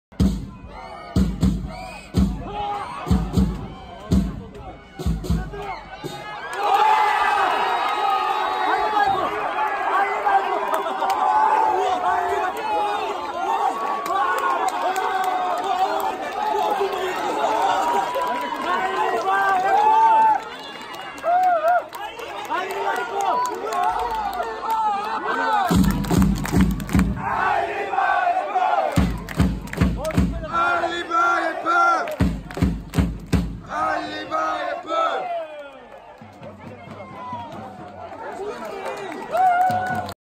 2019/5/19 FC서울 vs 상주상무 FC서울 알리바예프 골(득점) 직캠 + 알리바예프 선수콜 (FCSEOUL SUPPORTERS AWAYDAYS)
2019/5/19 FCSEOUL vs sangju sangmu

FCSEOUL NO.9 Ikromjon Alibaev scored cam + Alibaev call

FCSEOUL SUPPORTERS AWAYDAYS..